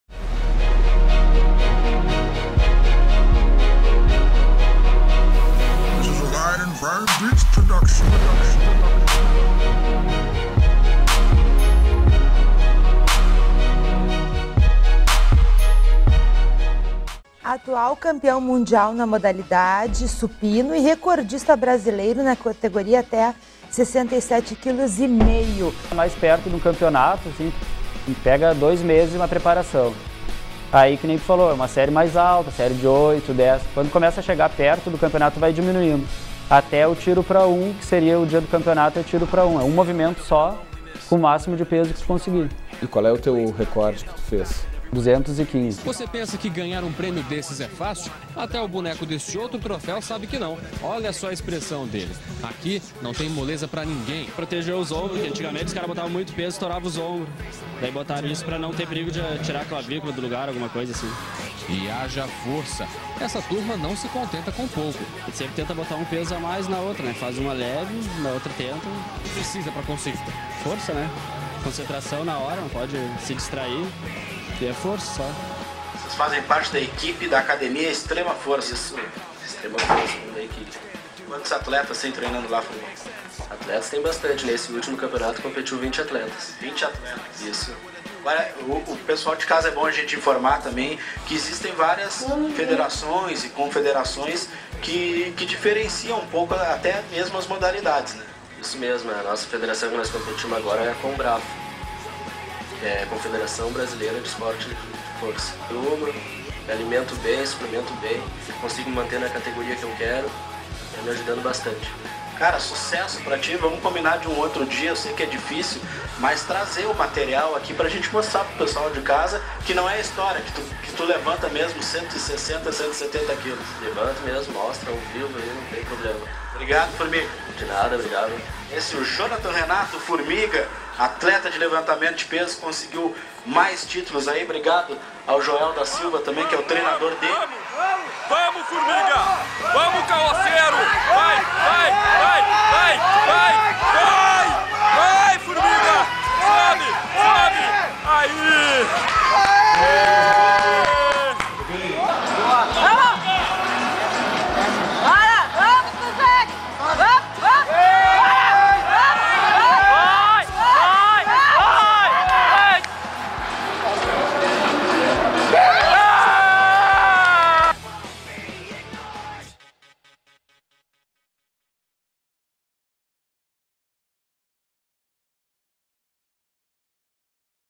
CFX Sports - Jonathan '' Formiga '' Machado - Campeão MUNDIAL SUPINO
Jonathan Machado '' Formiga '' campeão MUNDIAL por peso ( 67kg ) supino conseguindo erguer 215 kg no seu recorde pessoal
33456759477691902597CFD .